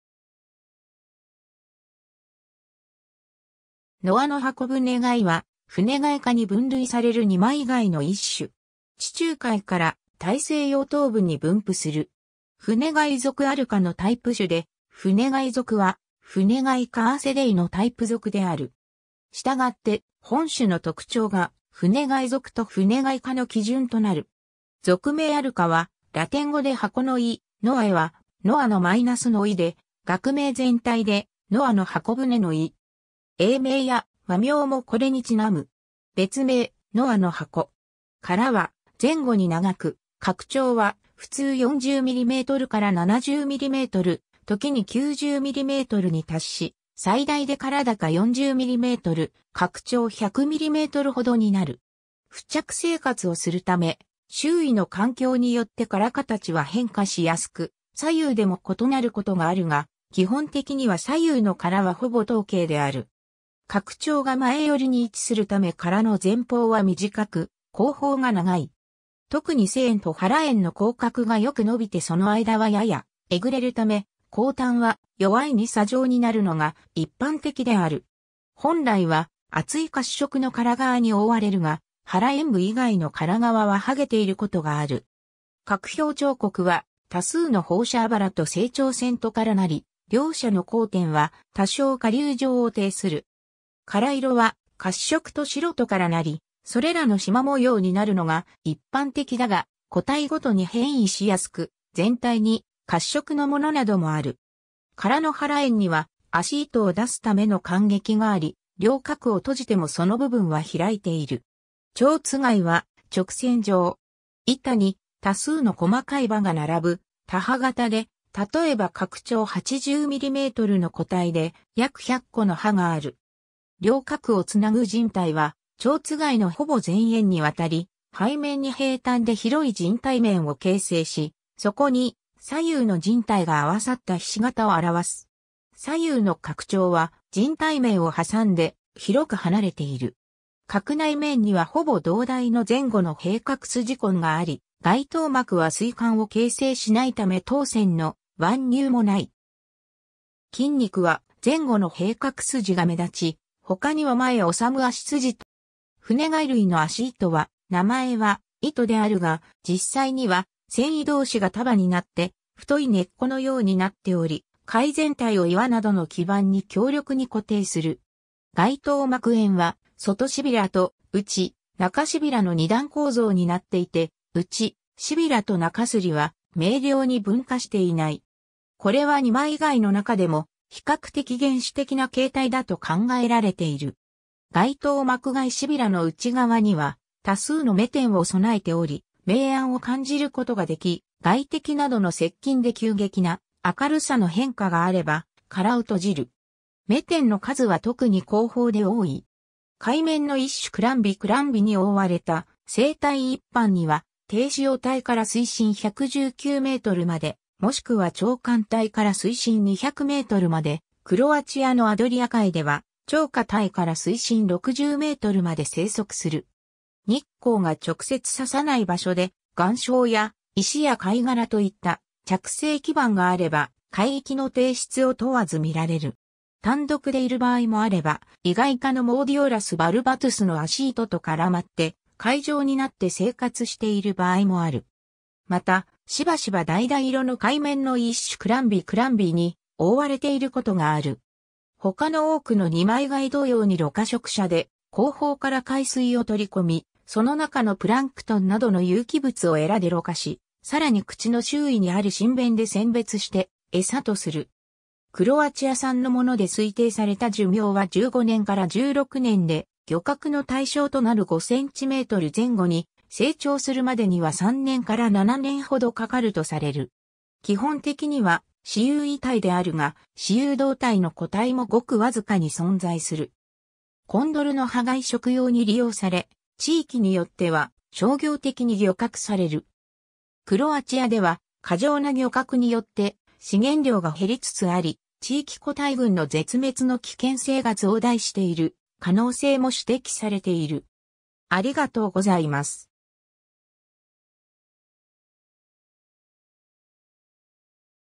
ノアノハコブネガイ
ノアノハコブネガイ（ノアの方舟貝、学名：Arca noae ）は、フネガイ科に分類される二枚貝の一種。
地中海から大西洋東部に分布する。
フネガイ属 Arca のタイプ種で、フネガイ属はフネガイ科 Arcidae のタイプ属である。
従って本種の特徴がフネガイ属とフネガイ科の基準となる。
属名 Arca はラテン語で箱の意、noae は「ノアの - 」の意で、学名全体で「ノアの方舟」の意。
英名や和名もこれに因む。
別名：ノアノハコ。
 殻は前後に長く、殻長は普通40mm-70mm、時に90mmに達し、最大で殻高40mm、殻長100mmほどになる。
付着生活をするため、周囲の環境によって殻形は変化しやすく、左右でも異なることがあるが、基本的には左右の殻はほぼ等形である。
殻頂が前寄りに位置するため殻の前方は短く後方が長い。
特に背縁と腹縁の後角がよく伸びてその間はややえぐれるため、後端は弱い二叉状になるのが一般的である。
 本来は厚い褐色の殻皮に覆われるが、腹縁部以外の殻皮は剥げていることがある。
殻表彫刻は、多数の放射肋と成長線とからなり、両者の交点は多少顆粒状を呈する。
殻色は褐色と白とからなり、それらの縞模様になるのが一般的だが、個体ごとに変異しやすく全体に褐色のものなどもある。
殻の腹縁には足糸を出すための間隙があり、両殻を閉じてもその部分は開いている。
 蝶番は直線状。
鉸板に多数の細かい鉸歯（蝶番の歯）が並ぶ多歯型で、例えば殻長80mmの個体で約100個の歯がある。
両殻を繋ぐ靭帯は蝶番のほぼ全縁にわたり、背面に平坦で広い靭帯面を形成し、そこに左右の靭帯が合わさった菱形を現す。
左右の殻頂は靭帯面を挟んで広く離れている。
殻内面にはほぼ同大の前後の閉殻筋痕（貝柱の痕）があり、外套膜は水管を形成しないため套線（外套膜の付着痕）の湾入もない。
 筋肉は、前後の閉殻筋（貝柱）が目立ち、ほかには前収足筋と後収足筋（収足筋＝足を殻内に引っ張る筋肉)、および巨大な後足糸牽引筋がそれぞれ1対ずつあるが、前足糸牽引筋はない。後足糸牽引筋の一端は後閉殻筋（後ろの貝柱）の前方に隣接して、殻内面の上部に付着し、他の一端は足糸の付け根に達していて、頑丈な足糸を牽引するようになっている。
フネガイ類の足糸は、名前は「糸」であるが、実際には繊維同士が束になって太い根っこのようになっており、貝全体を岩などの基盤に強力に固定する。
 外套膜縁は外褶と内／中褶の2段構造になっていて、内褶と中摺は明瞭に分化していない。
これは二枚貝の中でも比較的原始的な形態だと考えられている。
外套膜外褶の内側には多数の眼点を具えており、明暗を感じることができ、外敵などの接近で急激な明るさの変化があれば殻を閉じる。
眼点の数は特に後方で多い。
 カイメンの1種 Crambe crambe に覆われた生体 一般には低潮帯から水深119mまで、もしくは潮間帯から水深200mまで、クロアチアのアドリア海では潮下帯から水深60mまで生息する。
日光が直接射さない場所で、岩礁や石や貝殻といった着生基盤があれば海域の底質を問わず見られる。
単独でいる場合もあれば、イガイ科の Modiolus barbatus の足糸と絡まって塊状になって生活している場合もある。
また、しばしば橙色のカイメンの一種 Crambe crambe に覆われていることがある。
 他の多くの二枚貝同様に濾過食者で、後方から海水を取り込み、その中のプランクトンなどの有機物を鰓で濾過し、更に口の周囲にある唇弁で選別して餌とする。
クロアチア産のもので推定された寿命は15年-16年で、漁獲の対象となる5cm前後に成長するまでには3年から7年ほどかかるとされる。
基本的には雌雄異体であるが、雌雄同体の個体もごく僅かに存在する。
 コンドルノハガイ 食用に利用され、地域によっては商業的に漁獲される。
クロアチアでは過剰な漁獲によって資源量が減りつつあり、地域個体群の絶滅の危険性が増大している可能性も指摘されている（Peharda, 2002）。